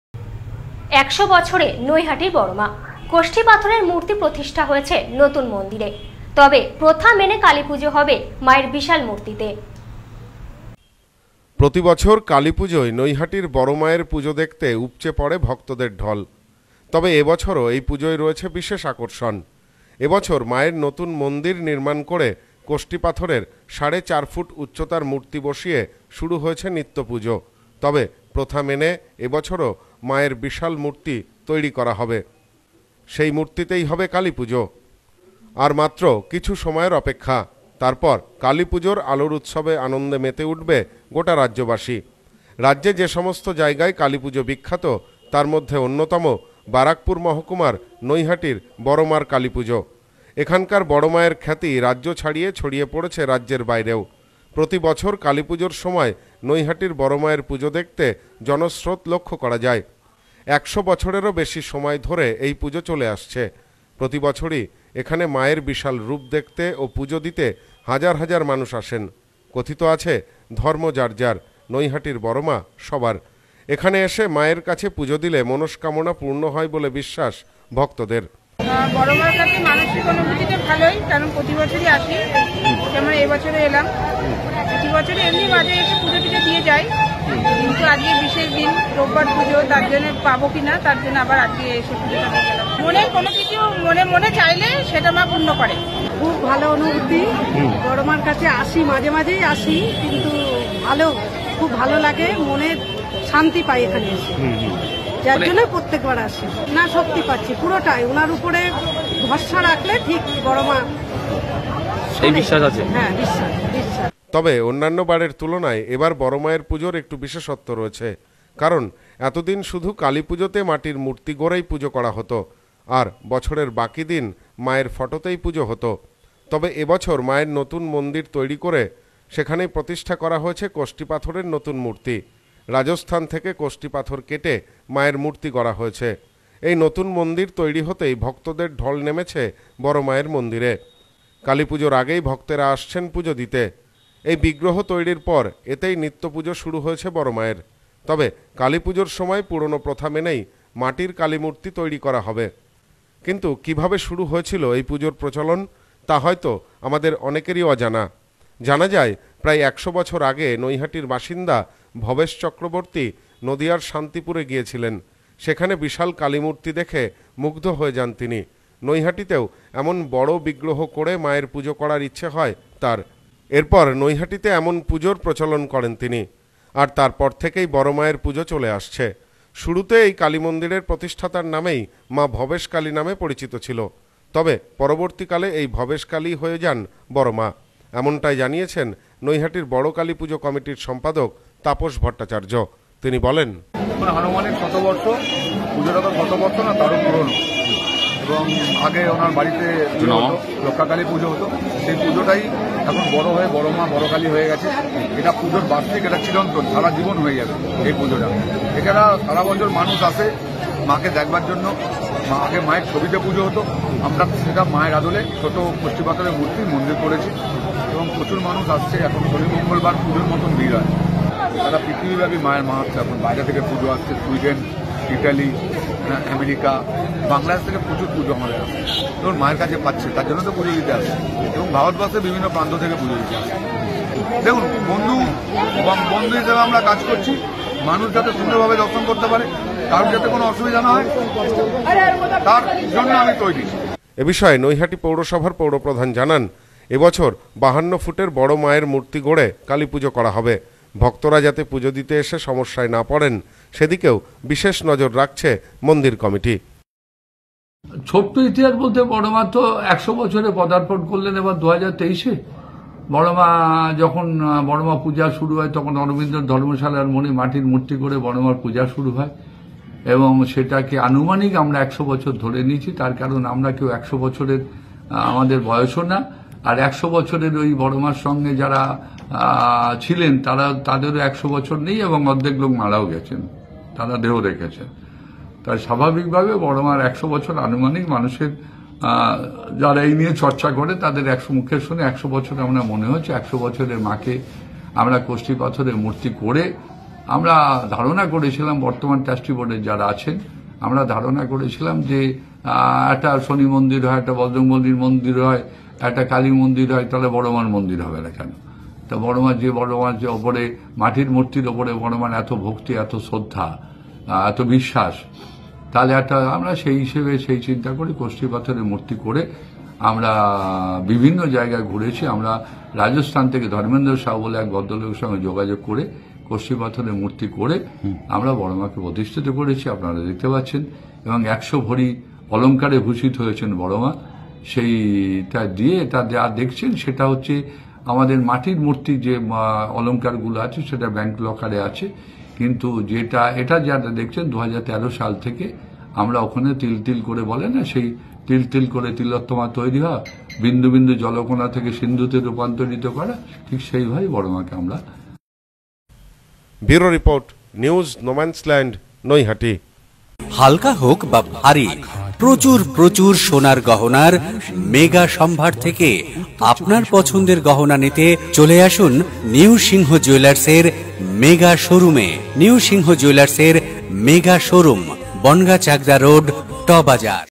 কষ্টিপাথরের মূর্তি নয় , কালী পুজোতে বিশাল মূর্তি গড়েই পূজিত হবে বড়মা।
১০০ বছরে নৈহাটির বড়মা। কষ্টি পাথরের মূর্তি প্রতিষ্ঠা হয়েছে নতুন মন্দিরে। তবে প্রথা মেনে কালীপুজো হবে মায়ের বিশাল মূর্তিতে।